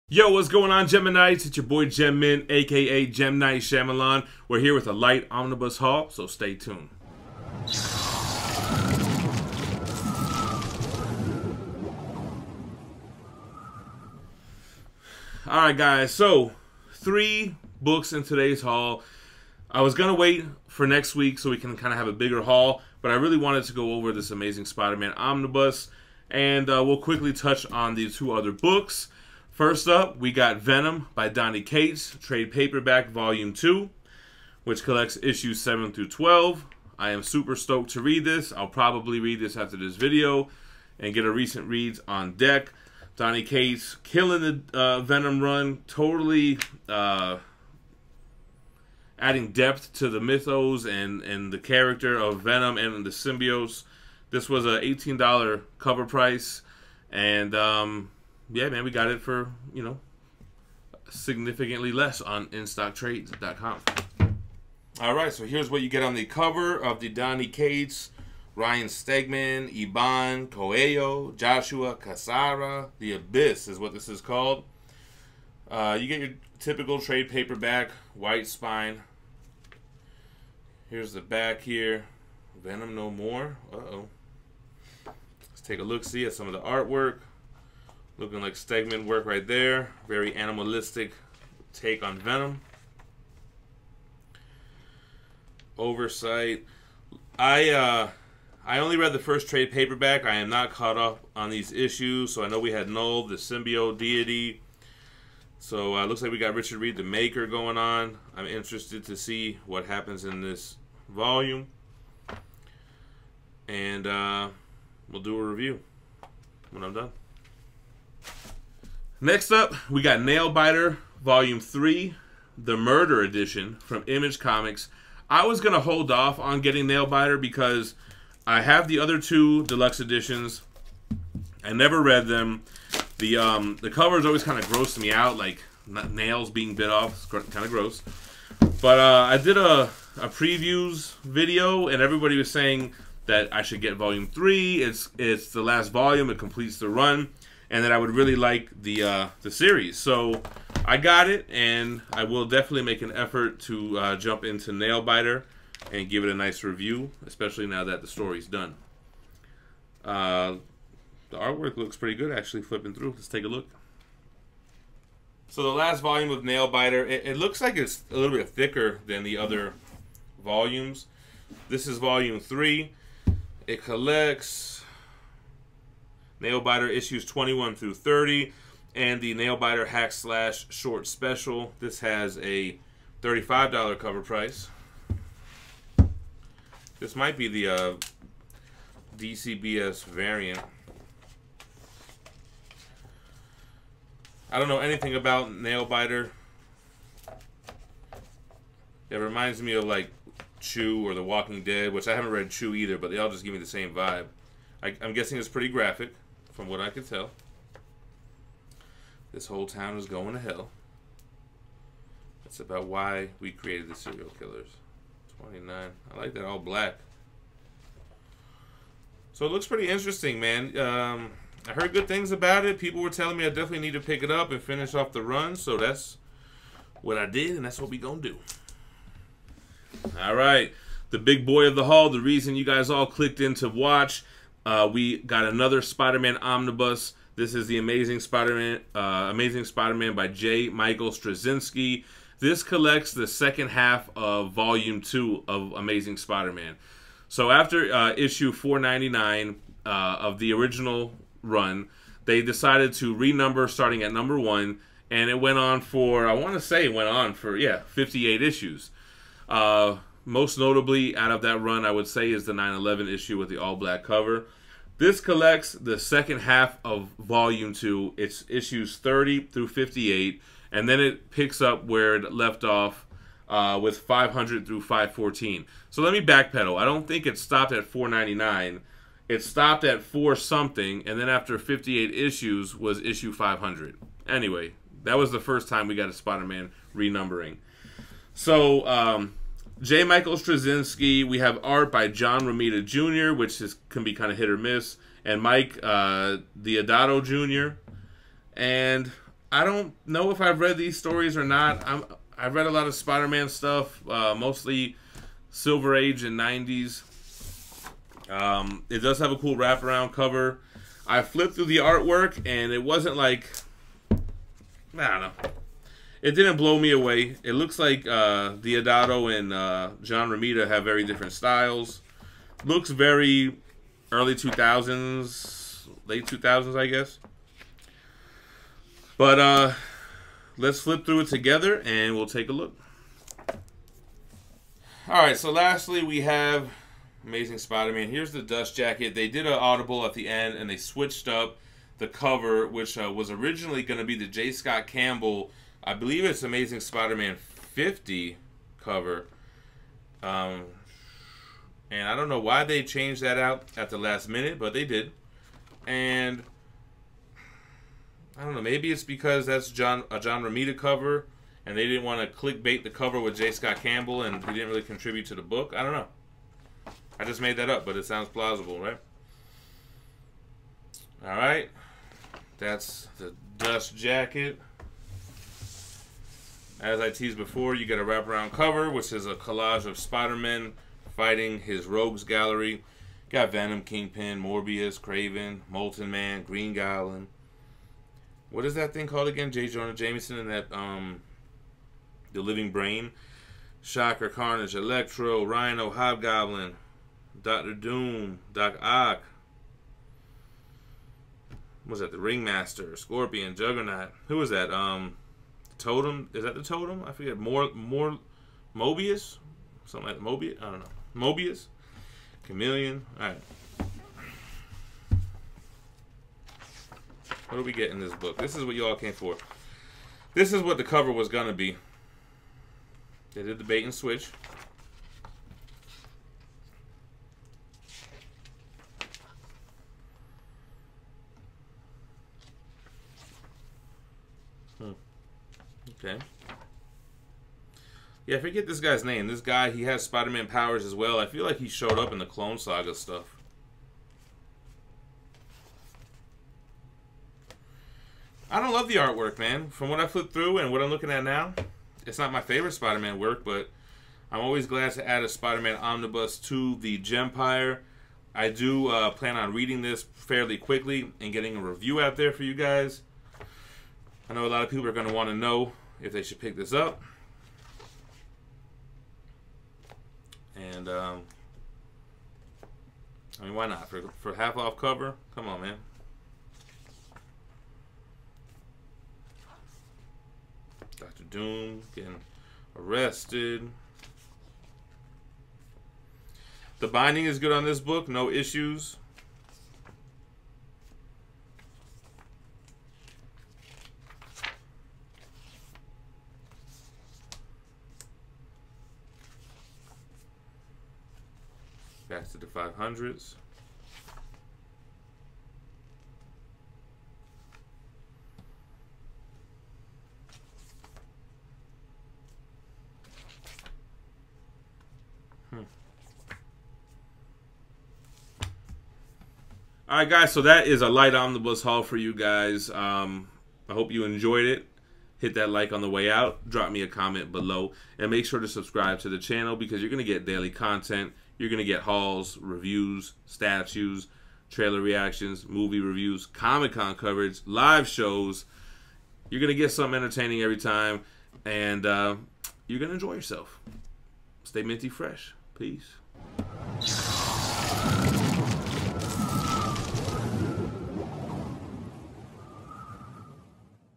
Omnibus COMIC BOOK Haul | Amazing Spider-Man | Venom | Nailbiter
A quick, 3 book haul: Venom vol 2 tpb by Donny Cates,  Nailbiter deluxe edition vol 3, and the Amazing Spider-Man Omnibus by Straczynski!